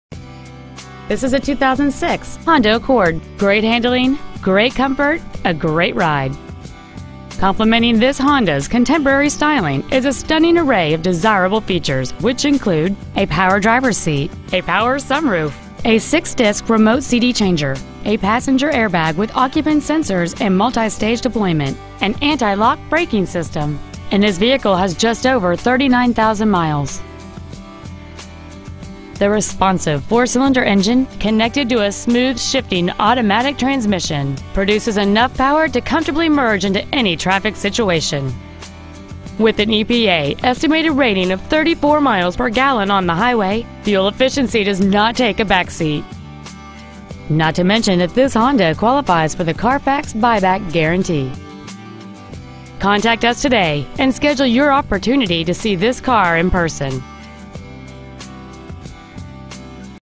Used 2006 Honda Accord San Jose CA 95129
Year: 2006
 Make: Honda
 Model: Accord
 Engine: 2.4L I-4cyl
 Trans.: Automatic
 Exterior: Bronze
 Miles: 39,145
 Interior: Beige